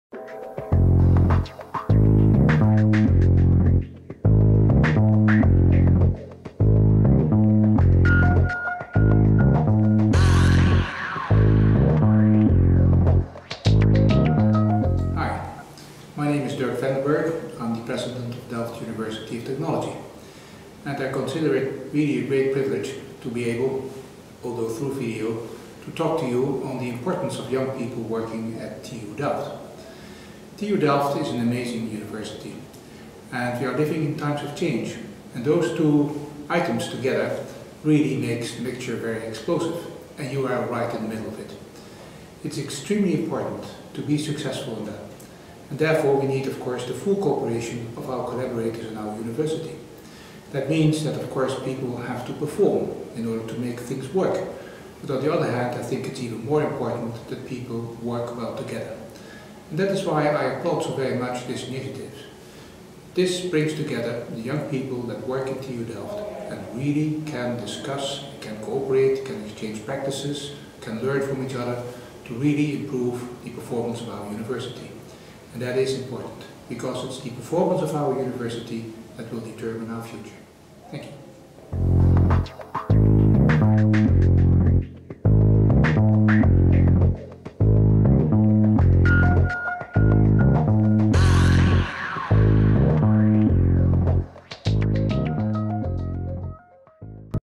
Dirk van den Berg
This video endorsement was shown at the Boost Camp 2011. In it Dirk Jan van den Berg, President of TU Delft, shares his vision on Jong Delft.